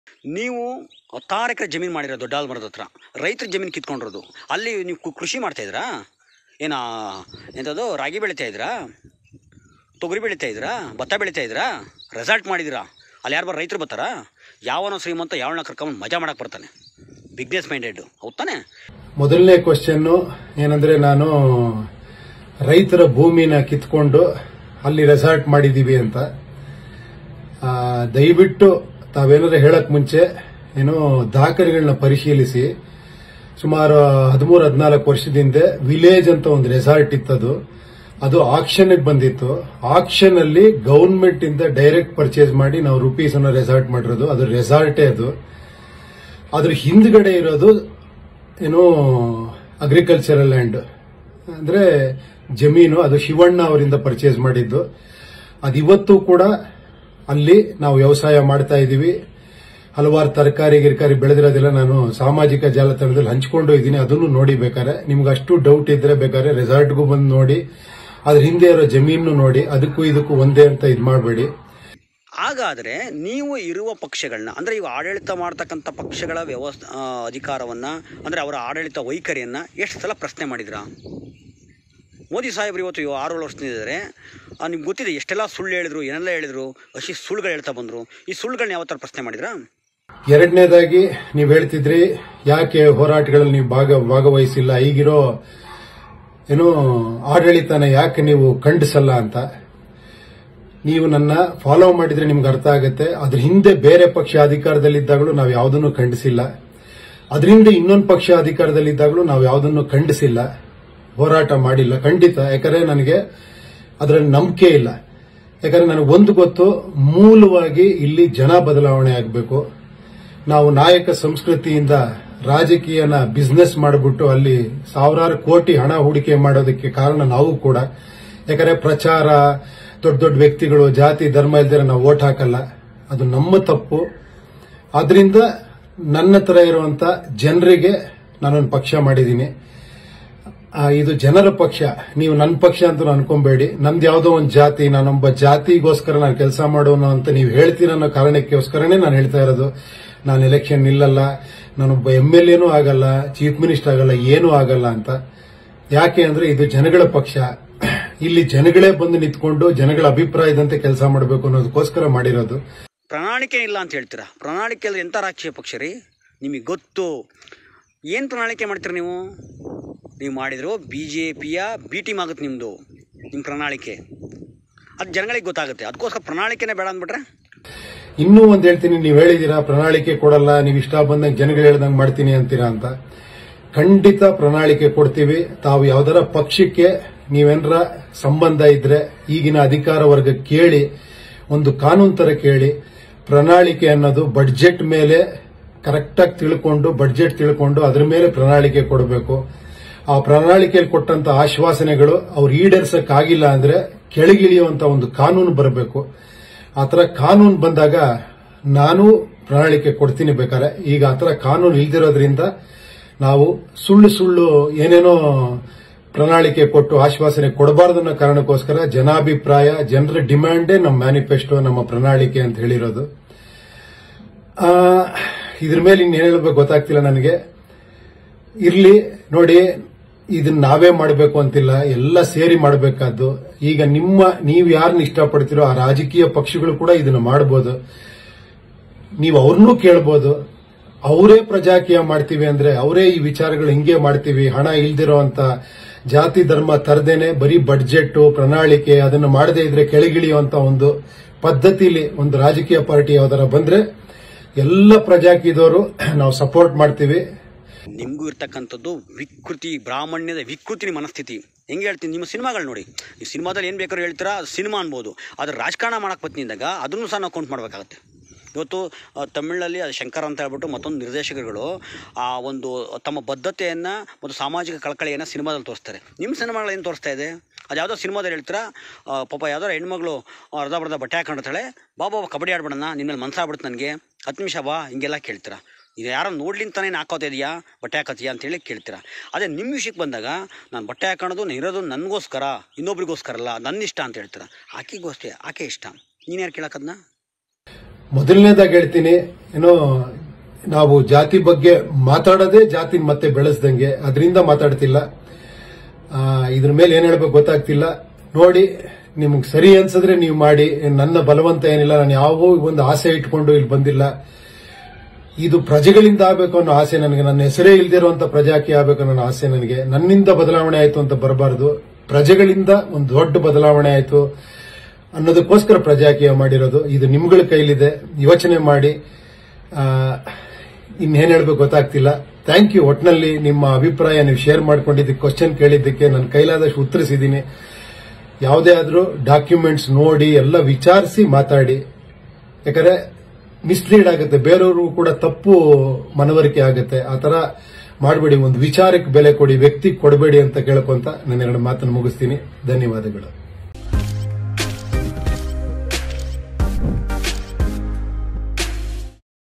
ತನ್ನ ಮೇಲಿರುವ ಎಲ್ಲಾ ಆರೋಪಕ್ಕೂ ಪ್ರತ್ಯುತ್ತರ ಕೊಟ್ಟ ರಿಯಲ್ ಸ್ಟಾರ್ Upendra | Oneindia Kannada
ರಿಯಲ್ ಸ್ಟಾರ್ ತಮ್ಮ ಉಪ್ಪಿ ರೆಸಾರ್ಟ್ ಅನ್ನು ರೈತರ ಭೂಮಿ ಕಿತ್ತುಕೊಂಡು ನಿರ್ಮಿಸಿದ್ದಾರೆ ಎನ್ನುವ ಆರೋಪ ಕೇಳಿಬಂದಿದೆ. ಈ ಬಗ್ಗೆ ನೆಟ್ಟಿಗನೊಬ್ಬ ವಿಡಿಯೋ ಮೂಲಕ ಉಪೇಂದ್ರ ಅವರಿಗೆ ನೇರ ಪ್ರಶ್ನೆ ಮಾಡಿದ್ದಾರೆ.ಈ ಬಗ್ಗೆ ರಿಯಲ್ ಸ್ಟಾರ್ ಉಪೇಂದ್ರ ಅವರೇ ವಿಡಿಯೋ ಮೂಲಕ ಸ್ಪಷ್ಟನೆ ನೀಡಿದ್ದಾರೆ. ಏನಾದರು ಹೇಳುವ ಮೊದಲು ದಯವಿಟ್ಟು ದಾಖಲೆ ಪರಿಶೀಲಿಸಿ ಎಂದು ಹೇಳಿದ್ದಾರೆ.

Real Star Upendra responses to his Uppi Resort controversy.